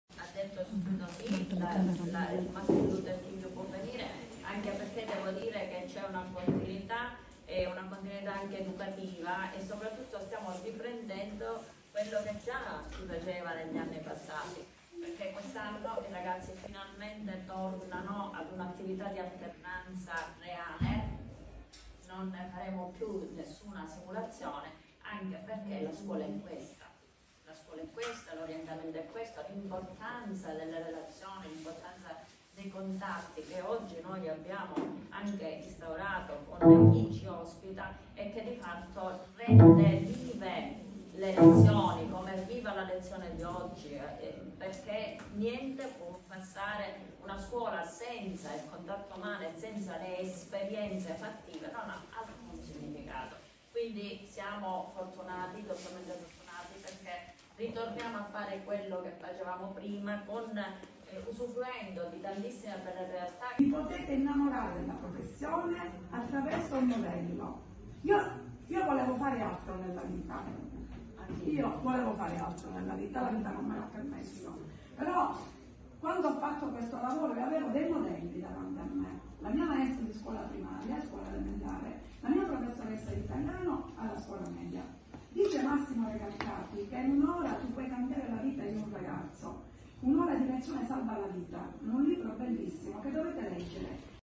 PCTO con L'IC Guarino
Gli allievi del Liceo Statale Martin Luther King diretto dalla dirigente scolastica prof.ssa Mirella Vella si apprestano a iniziare i loro percorsi per le competenze trasversali e per l'orientamento. Si è svolto presso la nostra scuola nell'aula Calogero Marrone il primo incontro con la responsabile del primo progetto la dirigente scolastica dell'istituto comprensivo Guarino la prof.ssa Gabriella Bruccoleri, gli alunni ed i docenti tutor delle classi del Liceo delle Scienze Umane : IV A docente tutor Rosalba Alaimo,
V A docente tutor Giusi Tuttolomondo, docente tutor III B Milena Iacolino
IV B docente tutor Elena Carrara, docente tutor V B Nadia Pullara.